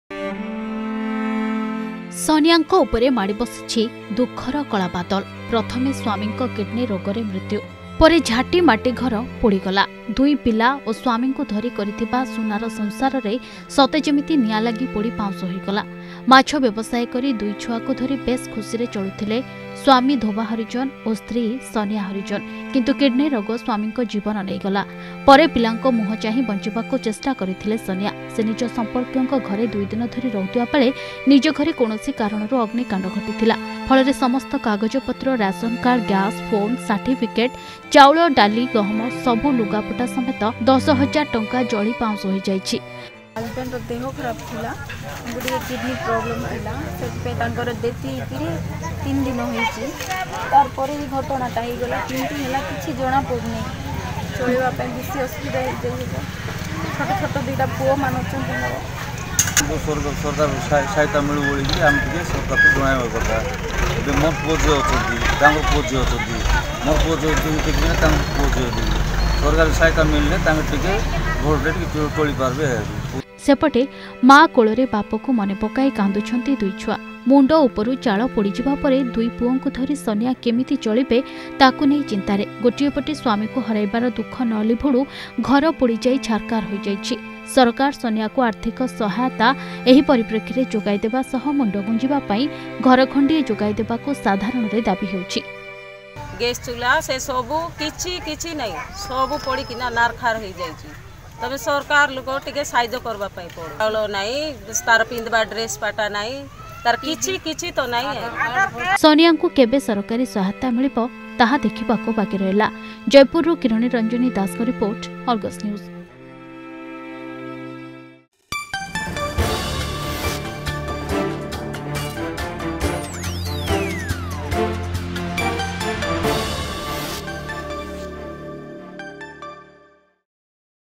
ଦୁଃଖ ଉପରେ ଦୁଃଖର ବୋଝ
ପ୍ରଥମେ ସ୍ୱାମୀଙ୍କ କିଡନୀ ରୋଗରେ ୩ ଦିନ ତଳେ ମୃତ୍ୟୁ ହୋଇଥିବା ବେଳେ ପରେ ଘର ପୋଡ଼ି । ସବୁ କିଛି ହୋଇଯାଇଛି ଛାରଖାର । ଫଳରେ ସନିଆ ଏବଂ ତାଙ୍କ ଛୋଟ ଛୋଟ ପିଲା ଅସହାୟ ହୋଇପଡିଛନ୍ତି । ଏପରି କୋହଭରା ଦୃଶ୍ୟ ଦେଖିବାକୁ ମିଳିଛି ଜୟପୁର ନିକଟସ୍ଥ ଜୟନଗର ...